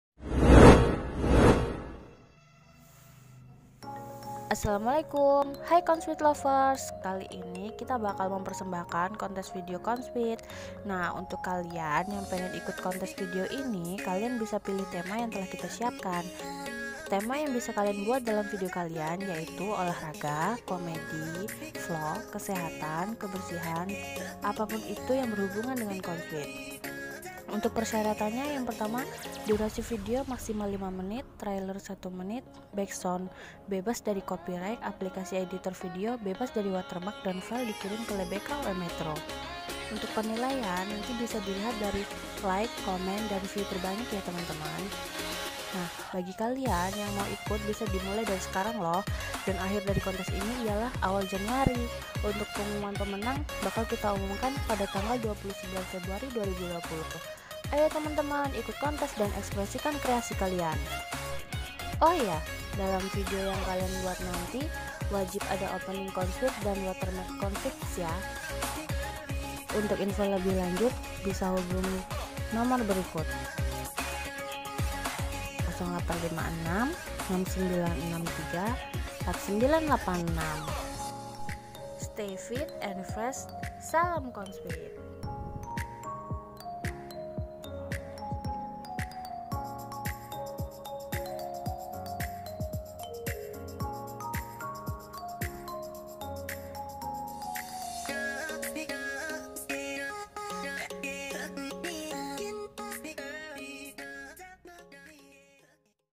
COUNSFIT VIDEO CONTEST
Assalamualaikum teman" semua.
*_COUNSFIT VIDEO CONTEST PRESENT_*
 Untuk yang ingin mengikuti kontes video di chanel counsfit di buka untuk umum dengan tema *olahraga, komedi, vlog, kesehatan,kebersihan dan yang berhubungan dengan counafit* persyaratan dibawah ini :
1. Durasi video *max 5 menit*.
2. Trailer 1 menit.
3. Backsound bebas dari copyright.
4. Apk editor video bebas dari watermark.
5. File di kirim ke lab bk um metro
6. Gunakan opening video counsfit dan watermark counsfit gaes!!!

ADA HADIAHNYA GAES !!! UNTUK JUARA 1, 2 dan 3 BERUPA UANG PEMBINAAN PEMBUATAN VIDEO DN SERTIFIKAT, KALAU JUARA HARAPAN 1,2 DAN Favorite HANYA SERTIFIKAT SAJA YA GAES.

Dimulai dari sekarang
Berakhir awal januari dan pengumuman  pemenang *29 februari 2020*

Mari berkreasi kawan"🙃